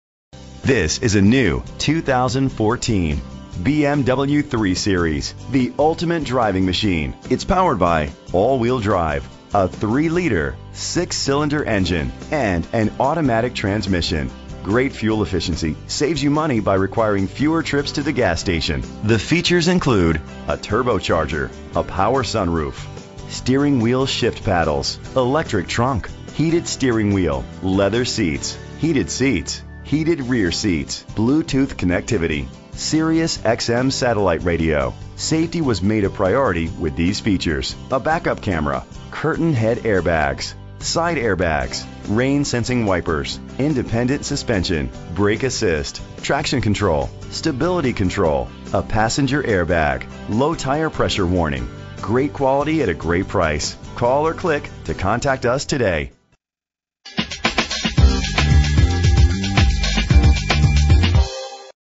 2014 BMW 3 Series - Elmhurst IL
Elmhurst BMW

Elmhurst BMW - BMW The Ultimate Driving Machine

Year: 2014
Make: BMW
Model: 3 Series
Trim: 335i xDrive
Engine: 3.0 liter inline 6 cylinder 24 valve
Transmission: 8-Speed Automatic
Color: Black Sapphire Metallic
Mileage: 10

Address:
466 West Lake St.